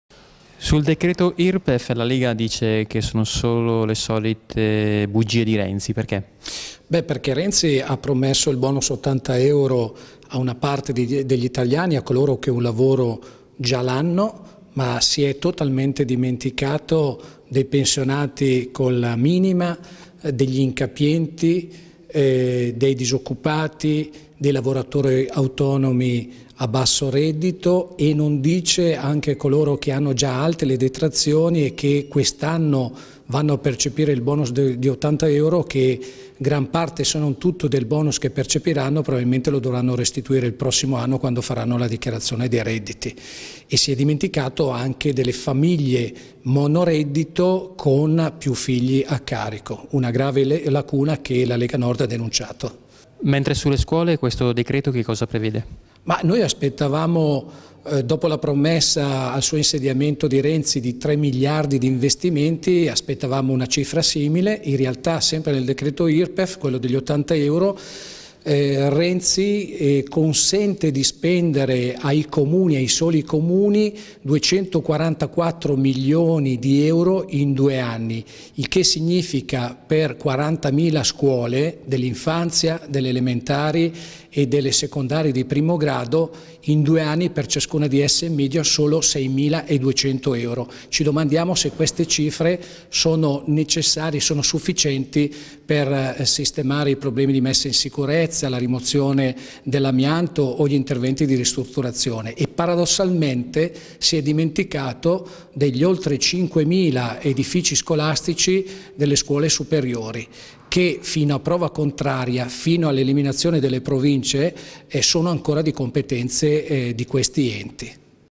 IRPEF: LEGA, SMASCHERATE BALLE RENZI. 80 PER POCHI PAGATI DA TUTTI E NIENTE PER TROPPI
ROMA, 4 GIU. - " L'approvazione in commissione bilancio di Palazzo Madama del provvedimento '80 euro' con la bocciatura di tutti i nostri emendamenti che chiedevano l'estensione della misura a incapienti, disoccupati, famiglie numerose con un solo genitore occupato e pensionati con la minima, sono la dimostrazione plastica delle balle di Renzi! I tecnici del senato ci avevano visto giusto, le coperture non ci sono e questa mancetta per pochi pagati da tutti con un aggravio della pressione fiscale non è nemmeno una misura strutturale. Questo esecutivo si dimostra completamente inadeguato ad affrontare la crisi e l'altissima disoccupazione e invece che allentare la morsa dell'austerità intrapresa da Monti prima e Letta poi Renzi prosegue nella stessa direzione aumentando le tasse".

Lo dichiarano i senatori della Lega Nord, Silvana Comaroli, Raffaela Bellot e Paolo Arrigoni.